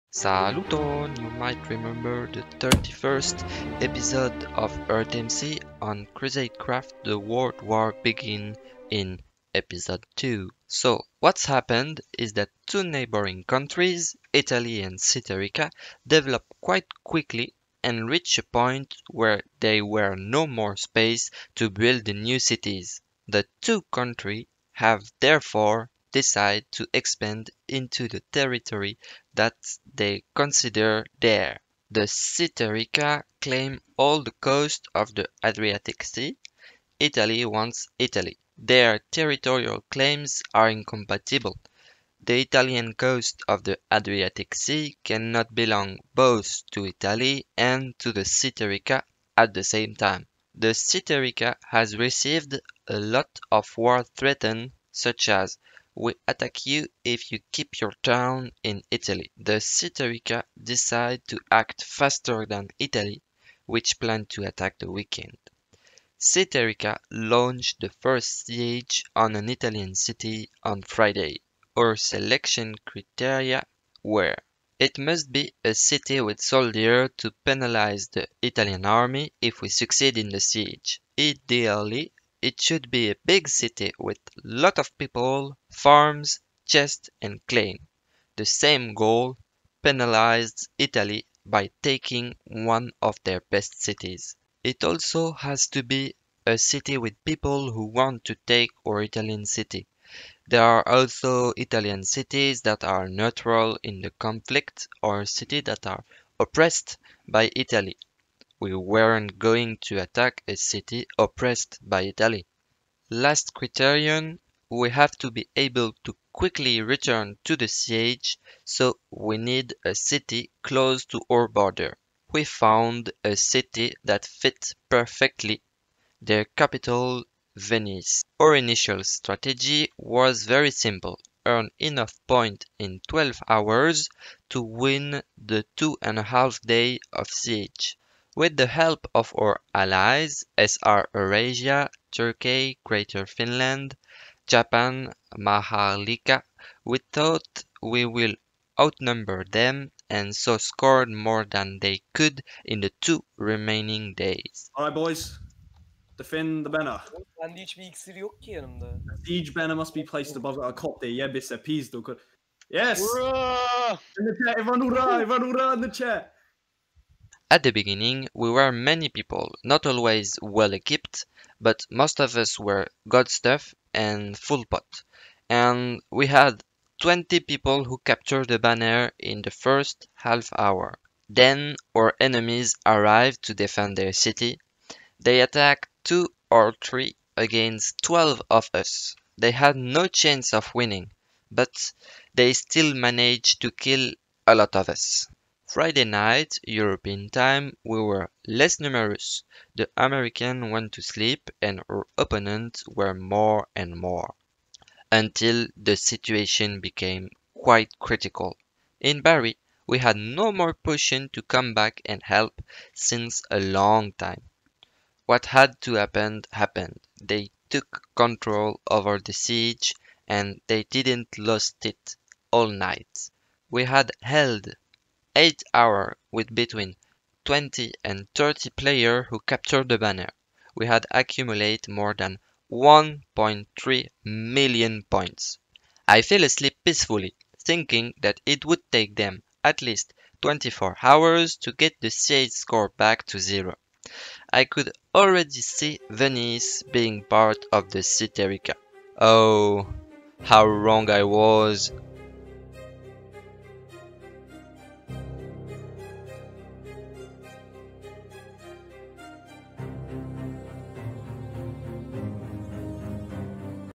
ep 2 ITALIA vs SEATERRICA / Part 1/3: Casus belli & 1st siege - Crusade Craft - cc (en)
The first part of the war against Italy!
The 2a part will probably arrive after Christmas so have fun with this 1.

Créateurs d'image que j'utilise courament : (Merci a eux)
Warrior555
Miss Loup
Foresty